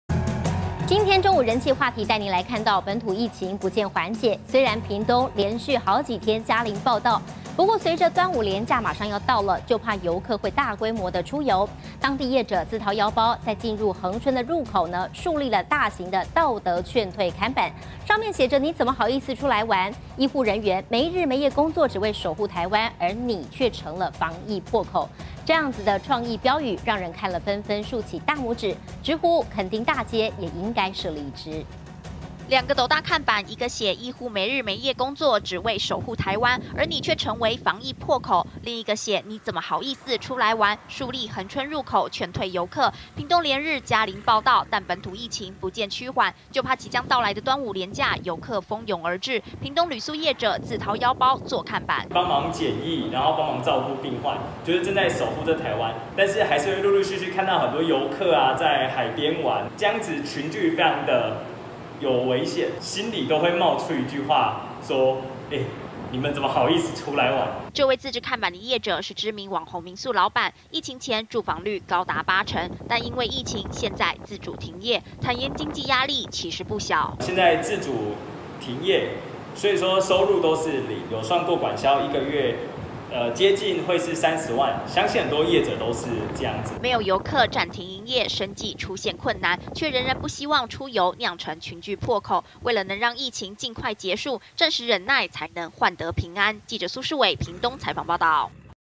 "醫護守護 你竟成破口" 道德勸退看板暴紅
本土疫情不見趨緩，雖然屏東連續數天加零報到，但隨著端午連假將至，就怕遊客大規模出遊，當地業者自掏腰包，在進入恆春的入口，豎立大型「道德勸退」看板，讓人看了紛紛豎起大拇指，直呼墾丁大街也該設立一支。